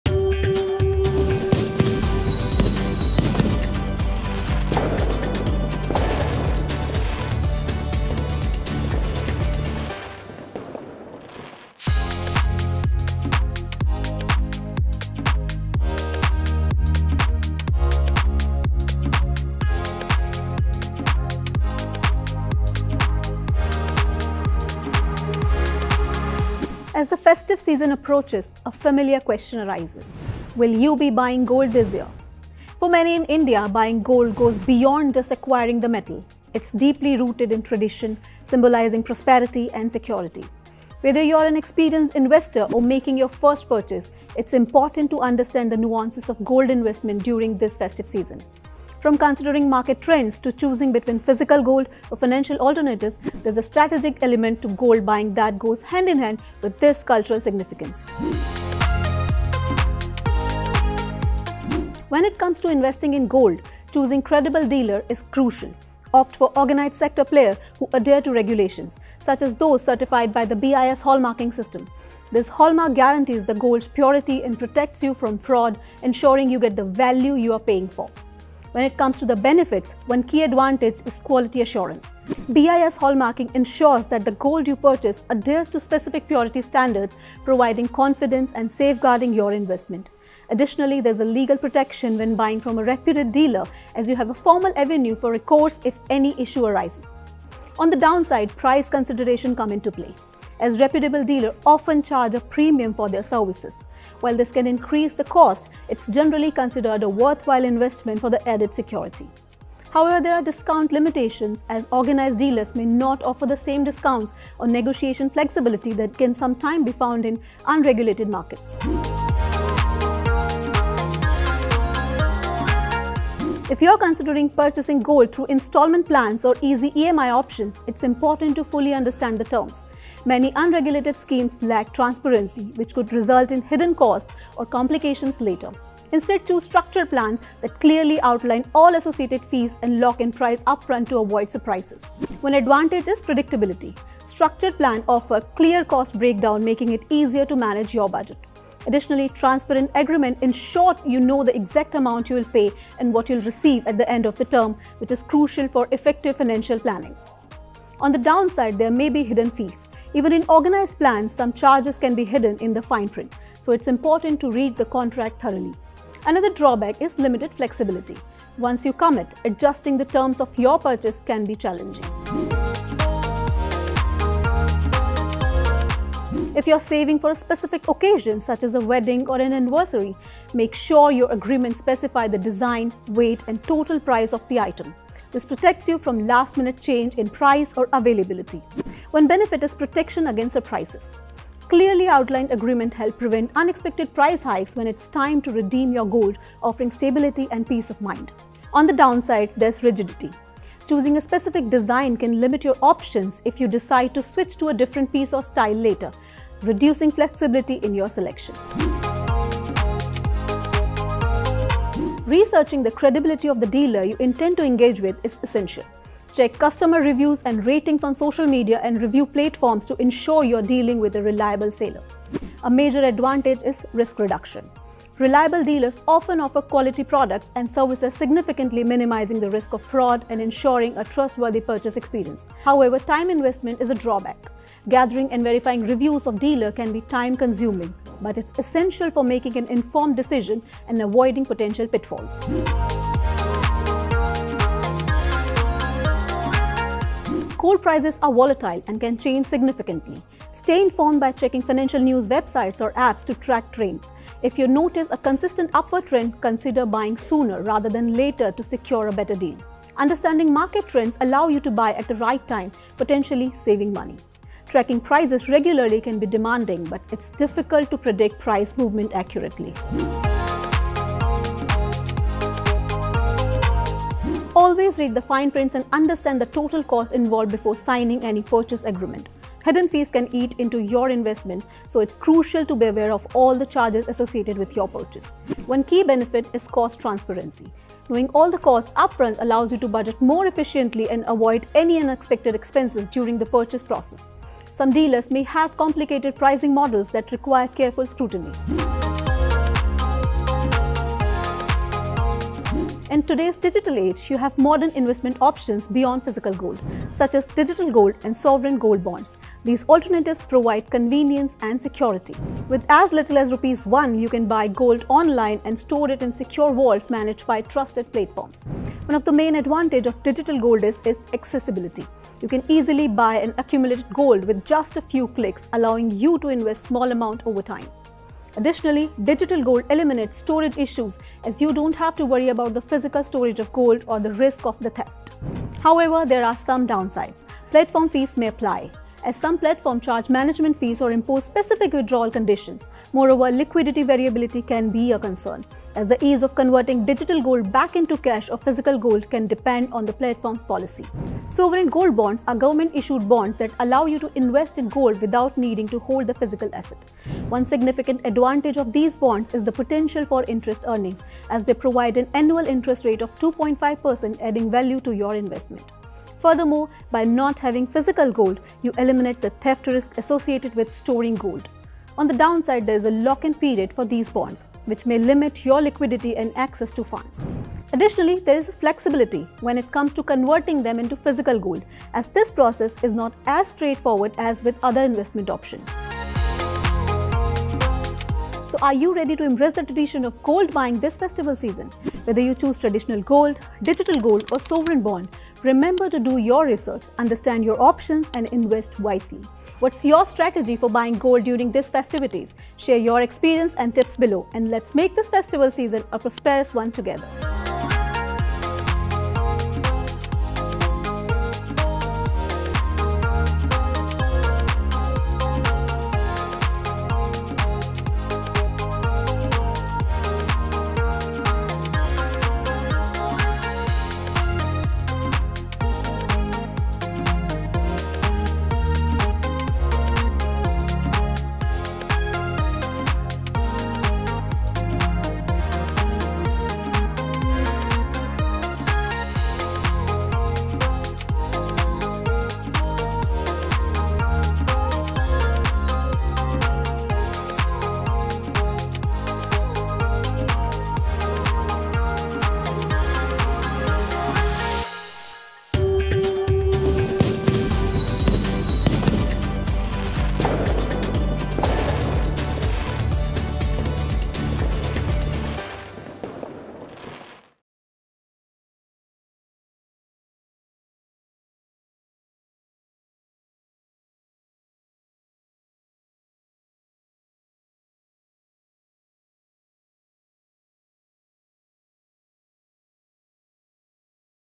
Smart Ways to Buy Gold This Festive Season | Essential Tips for Wise Investment | Dhanteras Special
Thinking of buying gold this festive season? Discover 6 smart ways to make a secure and profitable investment! From choosing trusted dealers to exploring digital gold and sovereign bonds, this guide covers all you need to know. Perfect for wedding plans or long-term savings, learn how to make informed choices that set you up for a prosperous festive season. Watch now to invest with confidence!